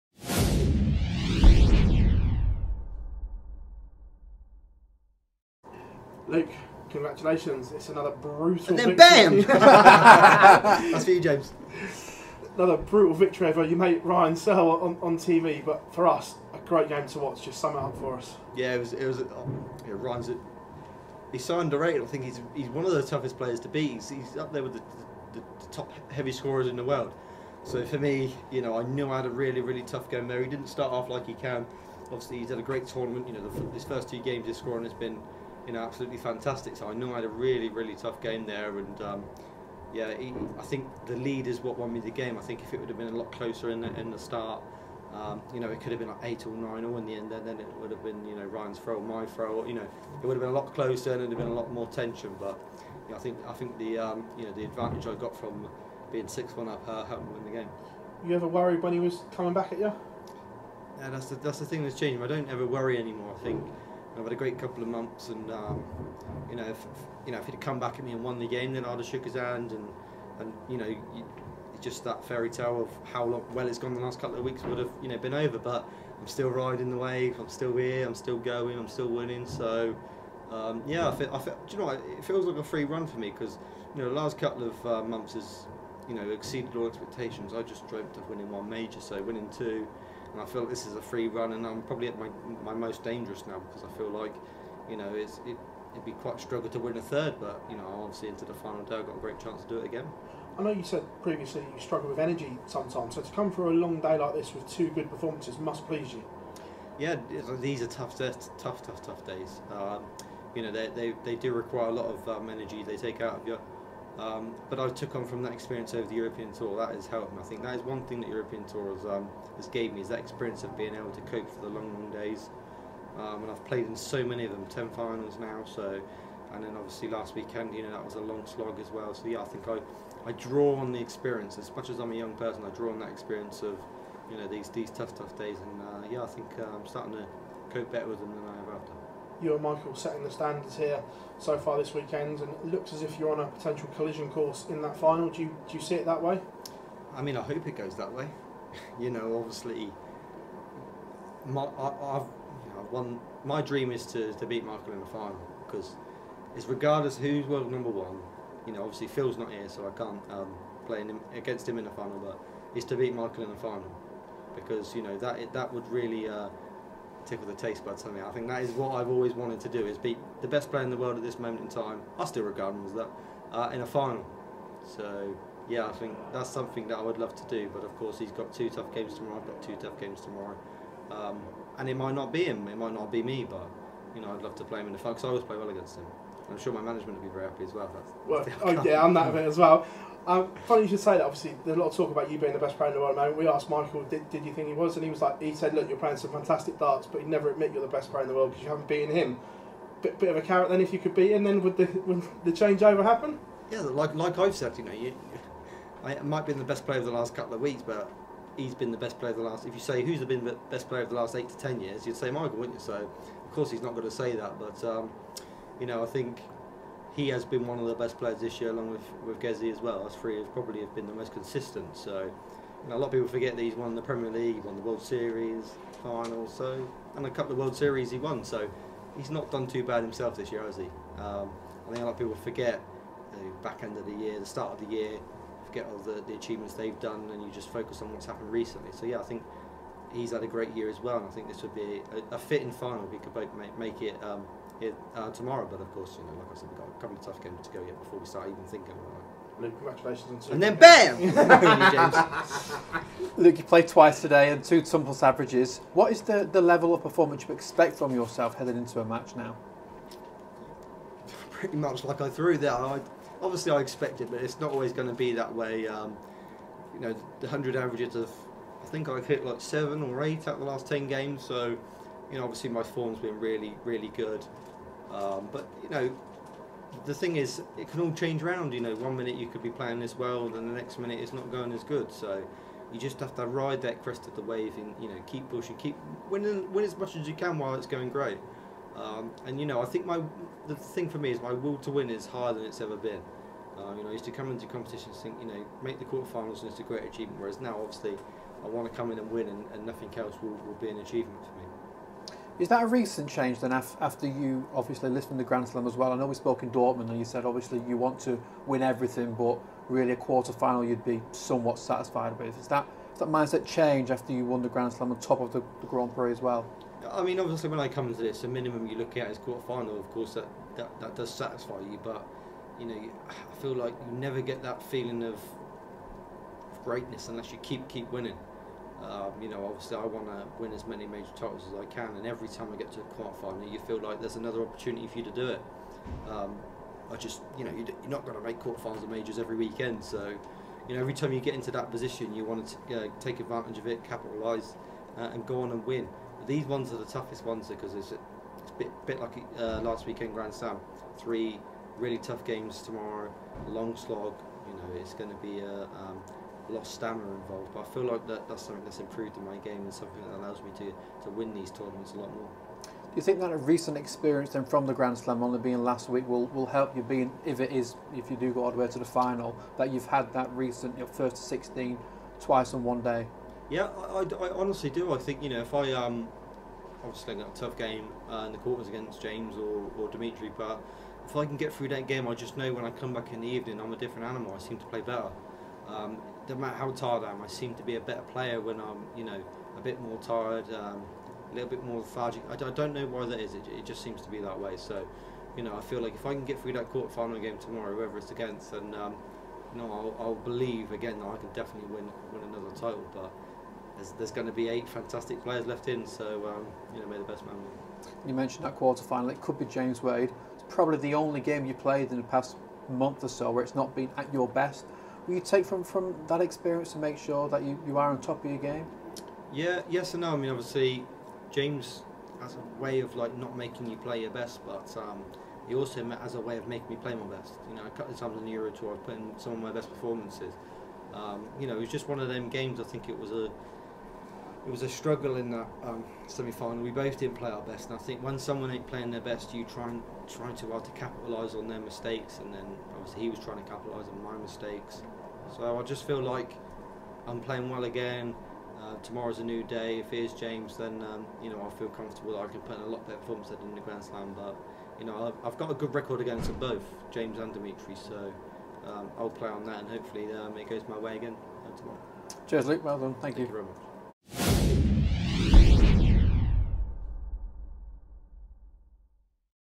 Luke Humphries DREAMS of beating MVG in a major final | “MVG is the BEST player in the world”
Luke Humphries says he dreams of beating MVG in a major final, and has a bit of banter with good friend James Wade